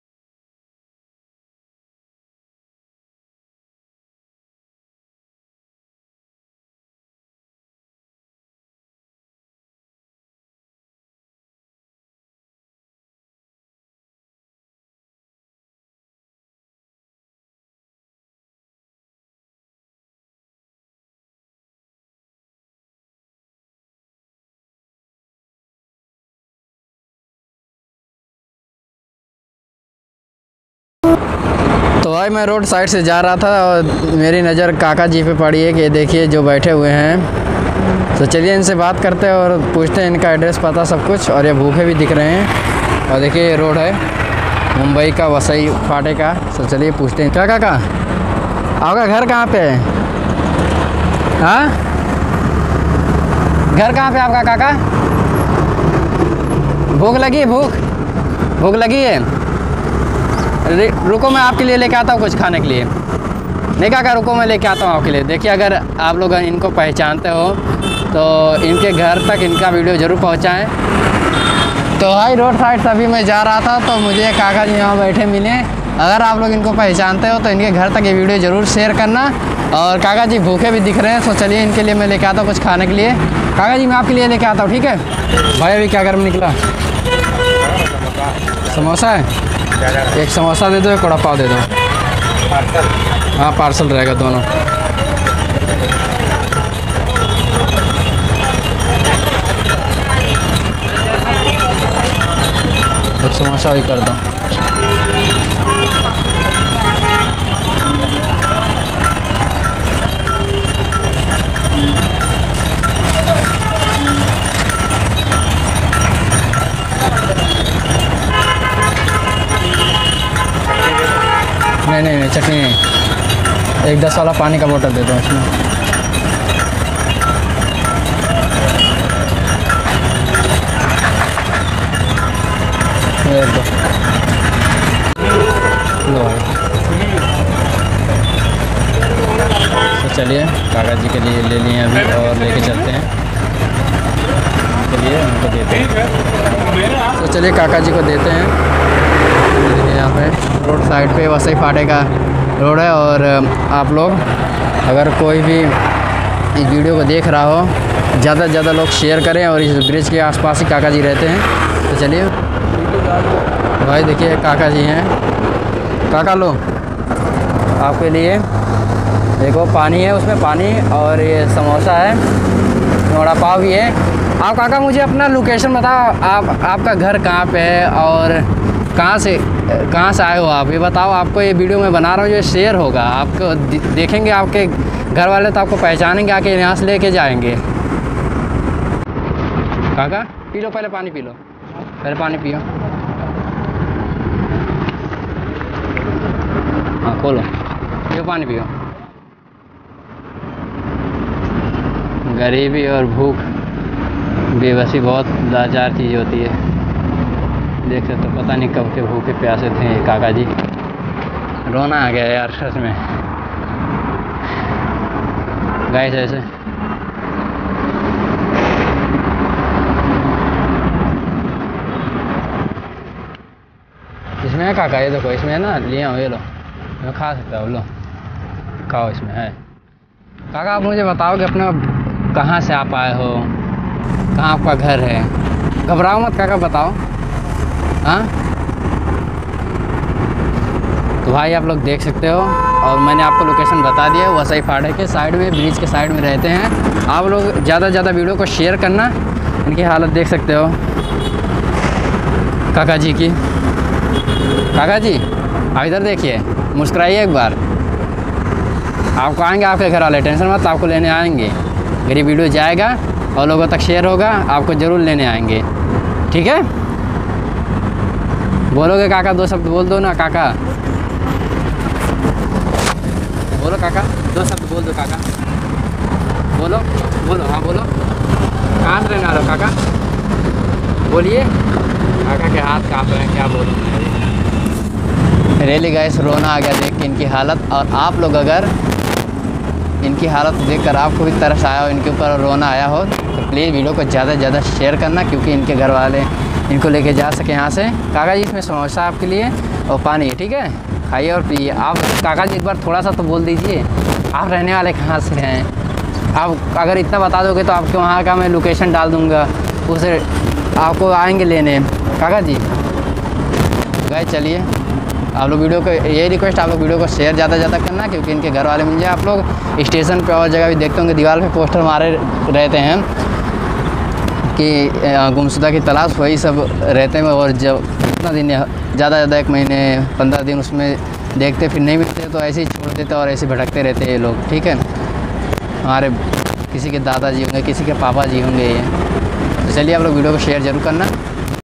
दादा जी की हालत देखकर आप रोने लगोगे 😭 | दिमागी हालत खराब है
दादा जी की हालत देखकर आप रोने लगोगे 😭 | दिमागी हालत खराब है | ‎@Help Them Foundation

दादाजी मुंबई में भटक रहे हैं इनकी मानसिक हालत खराब है | और पता नहीं किस
के दादाजी हैं | किस के पापा जी हैं | प्लीज इस वीडियो@ को ज्यादा से ज्यादा शेयर करें 🙏जिससे कि उनके घरवाले तक यह वीडियो पहुंचे और वह लोग आकर इनको यहां से ले जाएं मुंबई में वसई !